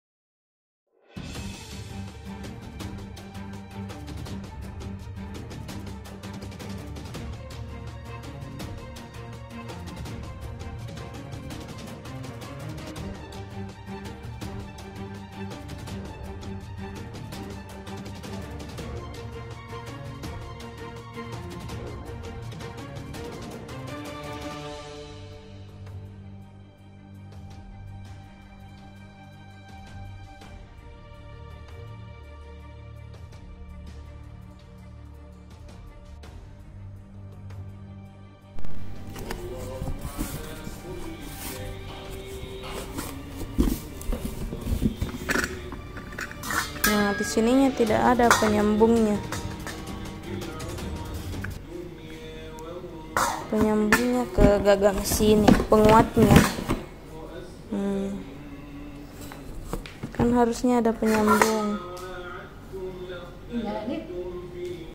PRODUK YANG DIKIRIM PENJUAL TIDAK LENGKAP DAN CACAT - KOMPLAIN DITOLAK PENJUAL - KOMPLAIN TOKOPEDIA
Saya order 3 barang yang sama, namun ada 1 barang yang saya order tidak lengkap, yaitu :

1. Pengait antara gagang dan spin (alat pelnya) tidak ada, JADI PERCUMA ! ALAT PELNYA TIDAK BISA DIGUNAKAN
2. Karet penutup bak, tidak rata dan berlubang (bocor).

saya lampirkan bukti tersebut dalam bentuk video dan foto.
catatan :
jika dibutuhkan bukti lainnya, saya siap mengirimkan video lainnya terkait komplain ini.

SEBELUM PENGIRIMAN, KAMI SUDAH TULIS DI CATATAN PENGIRIMAN (PIHAK TOKOPEDIA BISA CEK TRANSAKSI SAYA)

🔴 PENONTON TERHIPNOTIS SUARA ARIEL NOAH ❓INI YANG TERJADI SAAT LAGU BAND LAIN DINYANYIKAN ARIEL NOAH ~ Ariel NOAH Terbaru ~